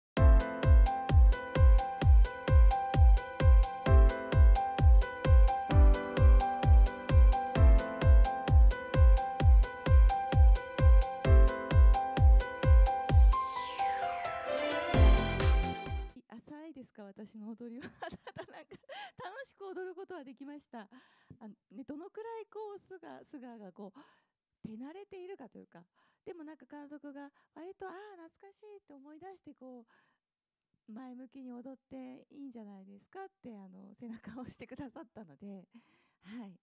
松たか子、透け感花柄ロングワンピース姿でエレガントに 映画「峠 最後のサムライ」公開御礼舞台あいさつ
女優の松たか子さんがこのほど、東京都内で行われた映画「峠 最後のサムライ」（小泉堯史監督）の公開御礼舞台あいさつに登場した。

　舞台あいさつには、主演の役所広司さん、共演の香川京子さん、田中泯さん、榎木孝明さん、AKIRAさんも場した。

　映画は、幕末の風雲児と呼ばれた越後長岡藩家老・河井継之助を描いた国民的ベストセラー「峠」（新潮文庫）初の映像化作品。慶応4（1868）年、鳥羽・伏見の戦いを皮切りに戊辰戦争が勃発。越後の小藩、長岡藩の家老・河井継之助（役所さん）は、東軍・西軍いずれにも属さない、武装中立を目指すが、談判は決裂。継之助は徳川譜代の大名として義を貫き、西軍と砲火を交えるという決断を下す……というストーリー。